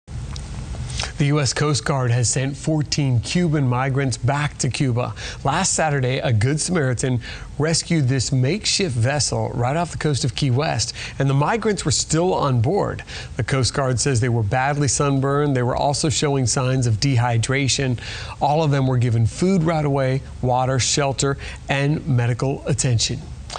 Coast Guard repatriates 14 migrants to Cuba
The Coast Guard repatriated 14 Cuban migrants to Cuba Tuesday. A good Samaritan reported the rustic vessel with 14 people aboard to Coast Guard Sector Key West watchstanders Saturday approximately 35 miles northwest of Key West.